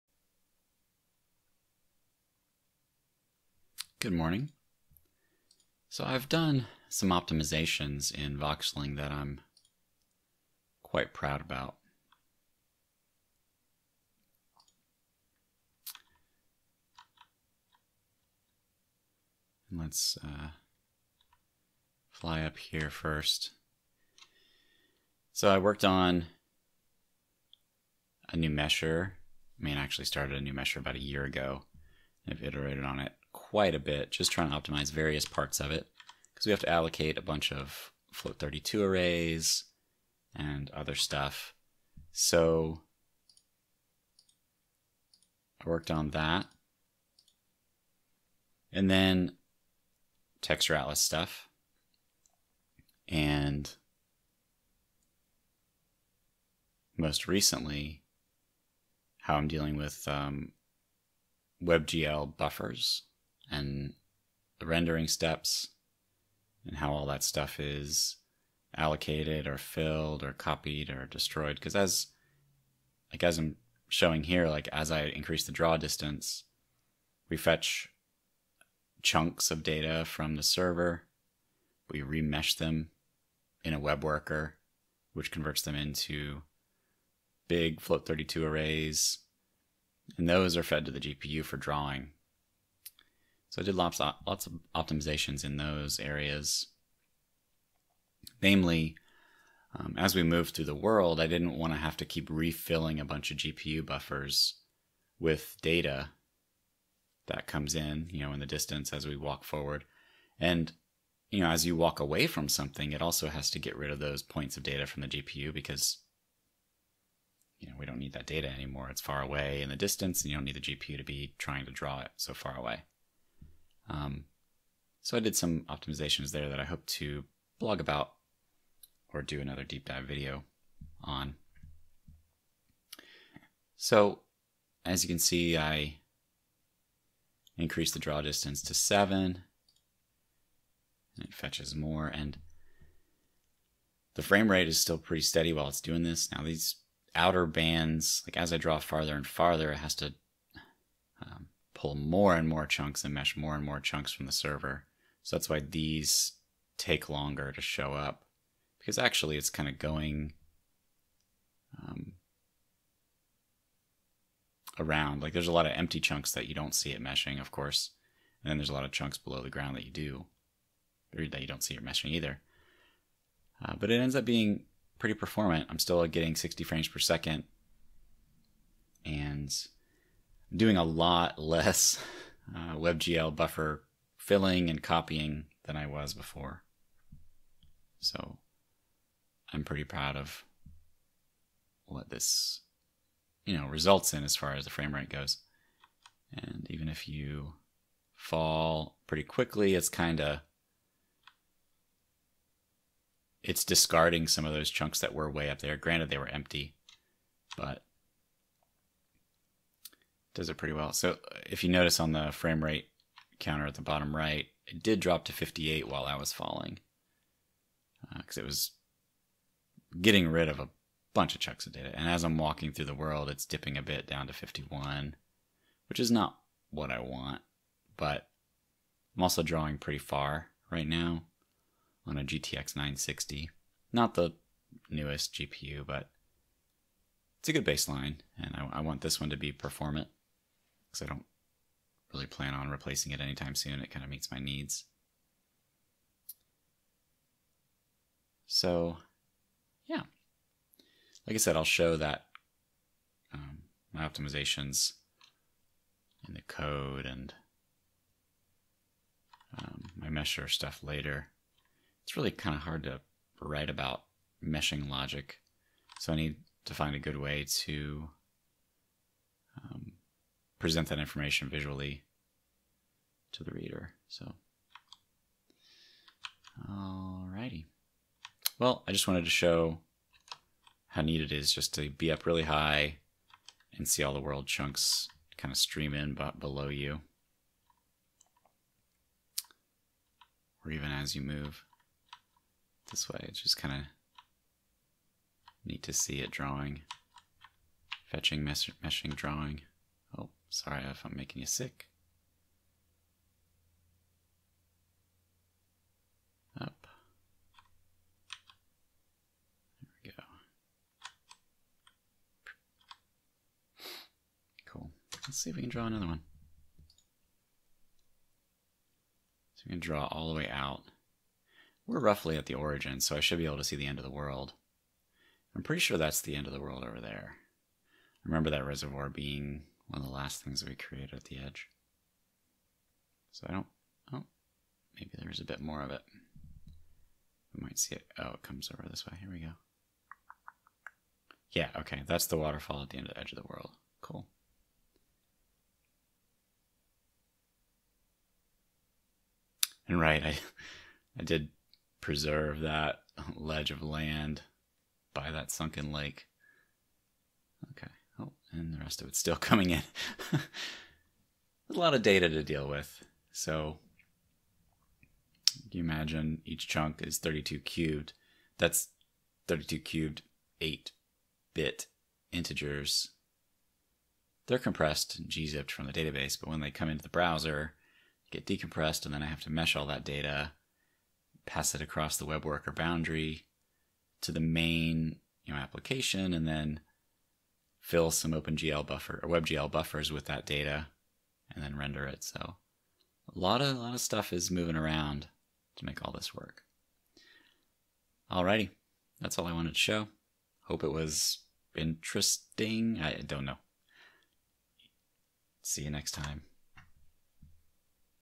Voxeling - 08 Many Optimizations
Showing what it feels like to move through the world and increase draw distance after many optimizations to meshing and WebGL buffer usage.